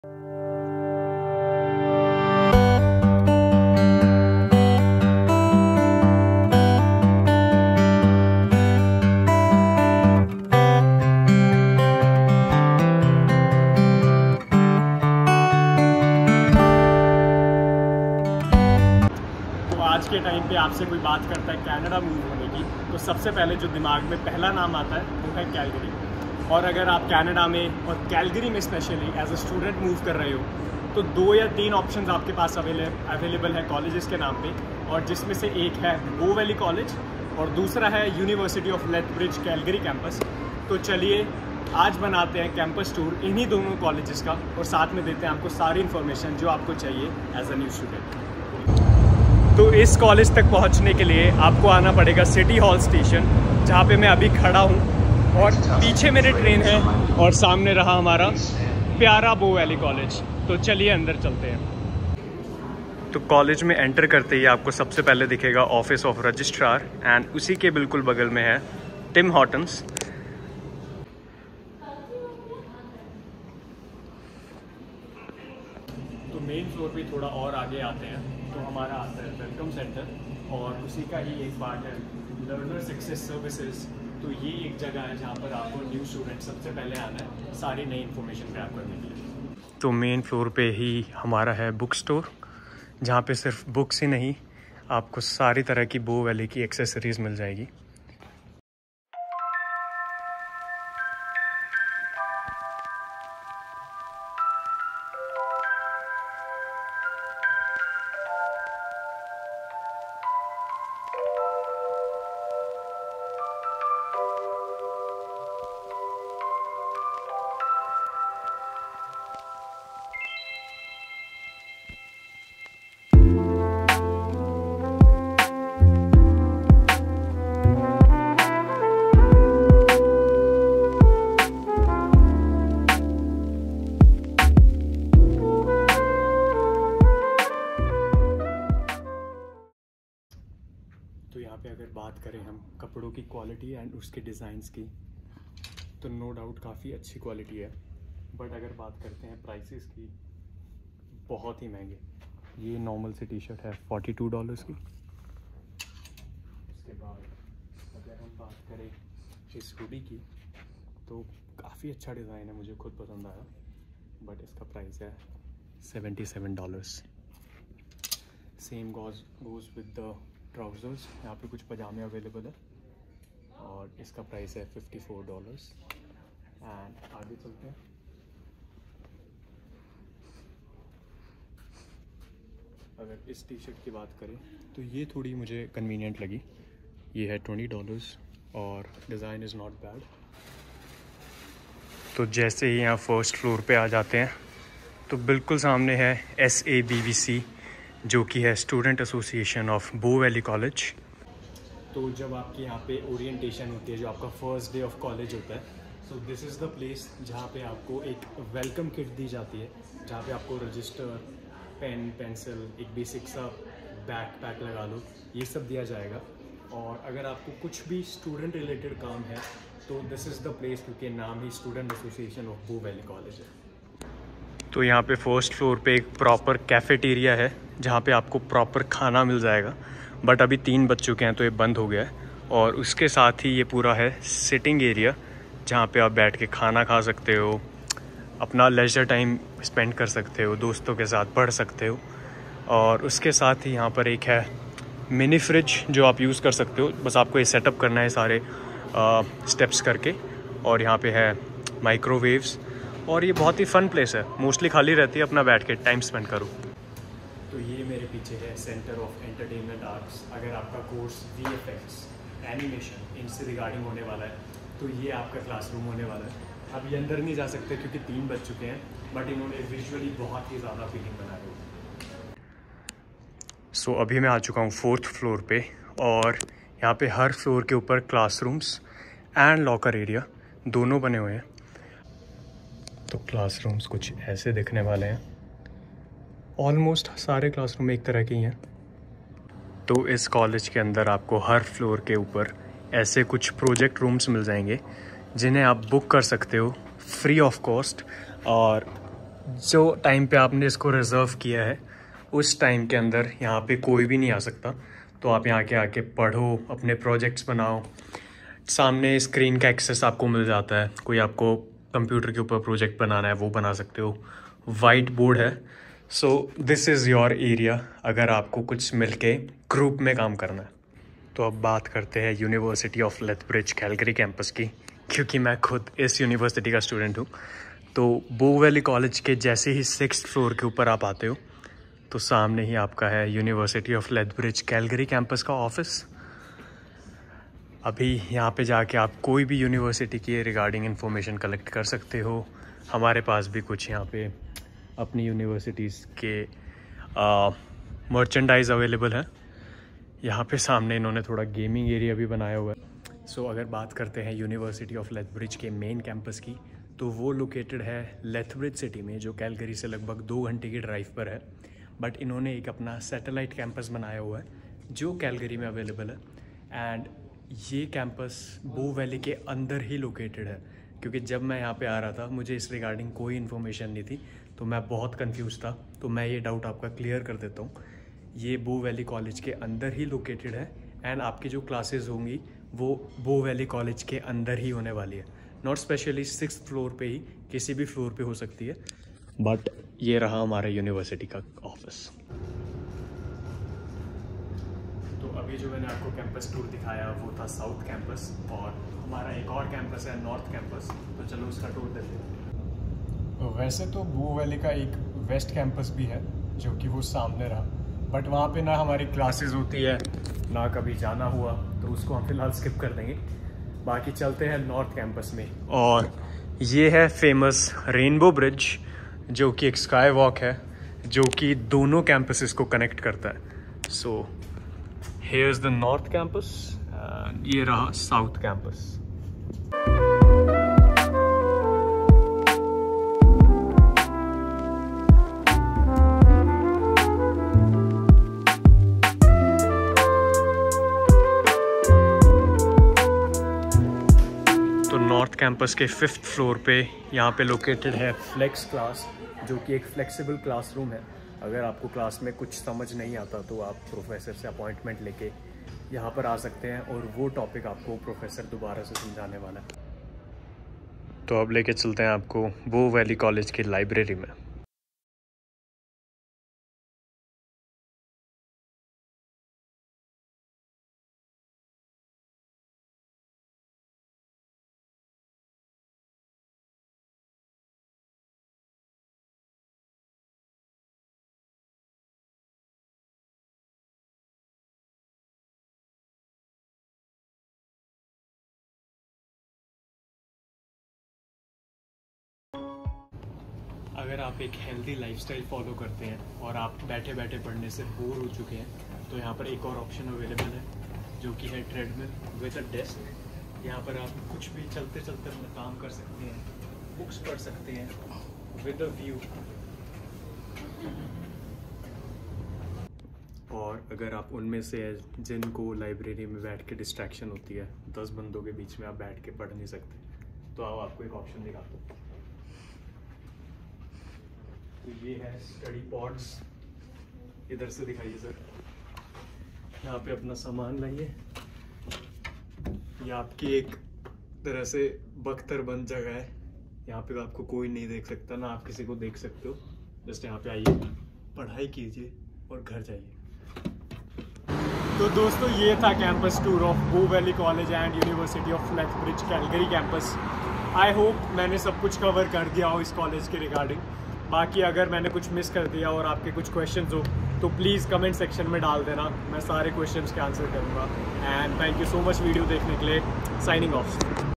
Campus Tour//Bow Valley College//University of Lethbridge (Calgary Campus).
Join us for an immersive tour of two prestigious institutions - Bow Valley College and the University of Lethbridge Calgary Campus